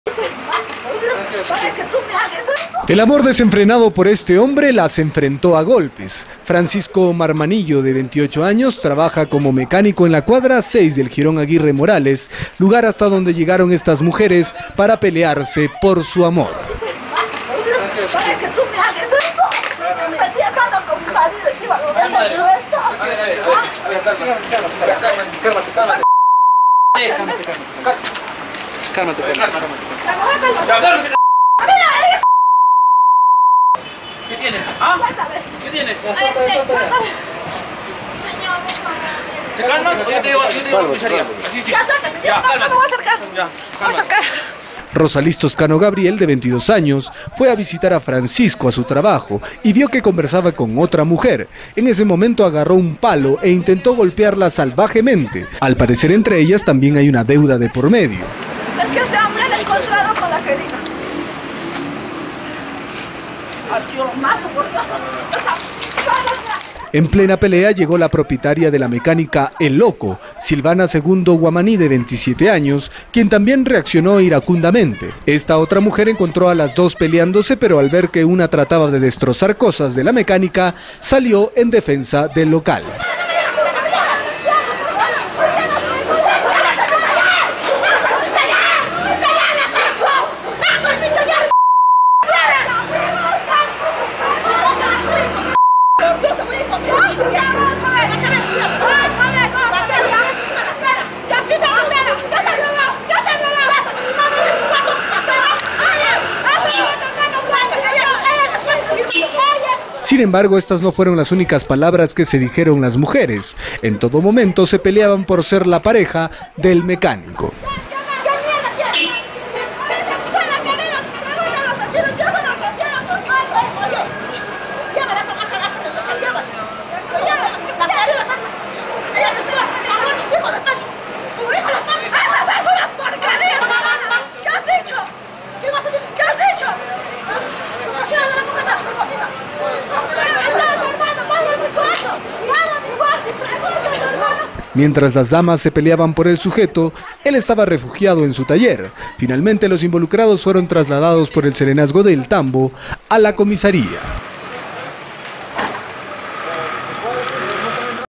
PELEAN POR AMOR DE MECÁNICO "DON JUAN" KDNA NOTICIAS
Tres mujeres armaron escándalo en taller donde el sujeto permanecía escondido.